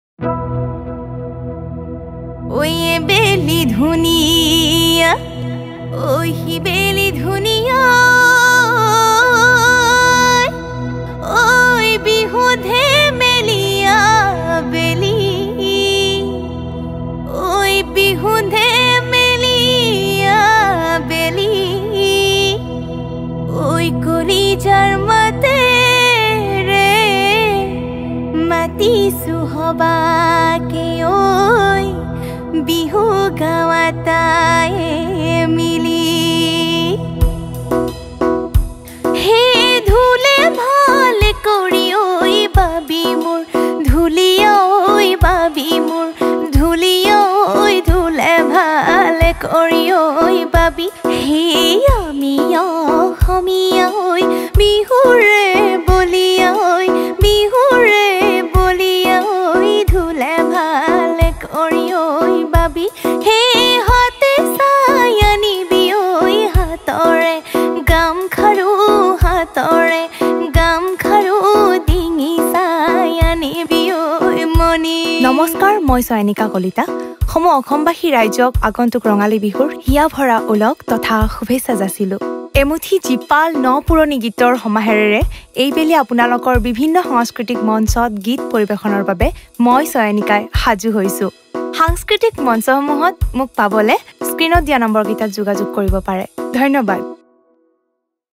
Bihu Song Mashup by Chayanika Kalita
Singer - Chayanika Kalita
Video Editing - Mamoni Kalita
Cemera - Nipankar Photo Factory
Recorded Studio - Play Hub
Special Thanks - Dharmananda Kalita,Mamoni Kalita & Bhumindra Das